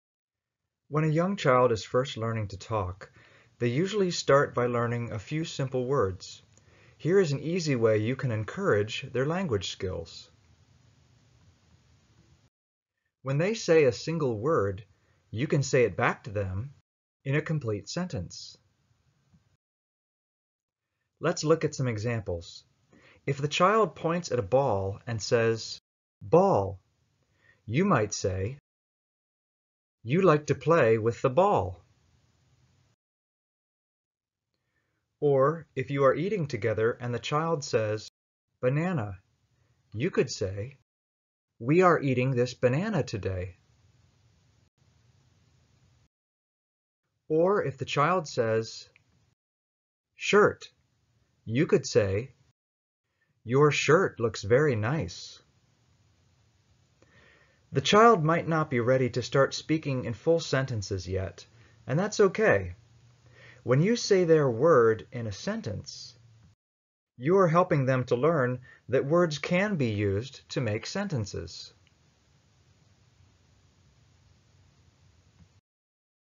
Learning that Words Make Sentences
Here is a simple activity you can use to help build a child's language skills. With practice they will begin to understand that word's can be used to make sentences.